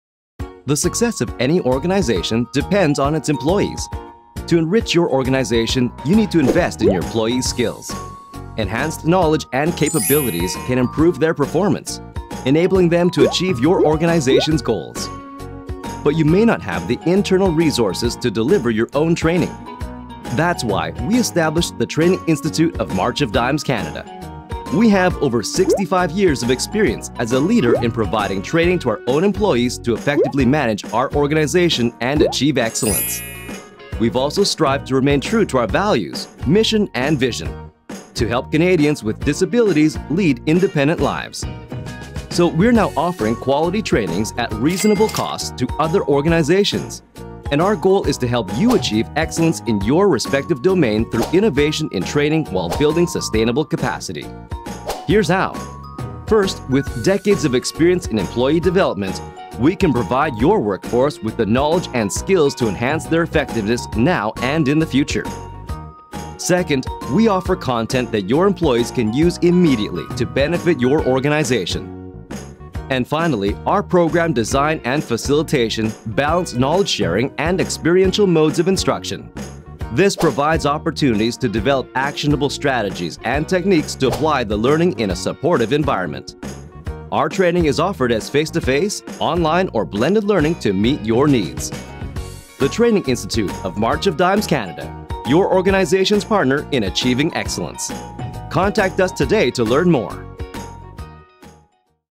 March of Dimes Canada's Training Institute
Did you know we have our very own Training Institute?

The Training Institute maximizes opportunities for leaders, managers and service providers to achieve excellence in their respective domains through innovation in training while building sustainable capacity.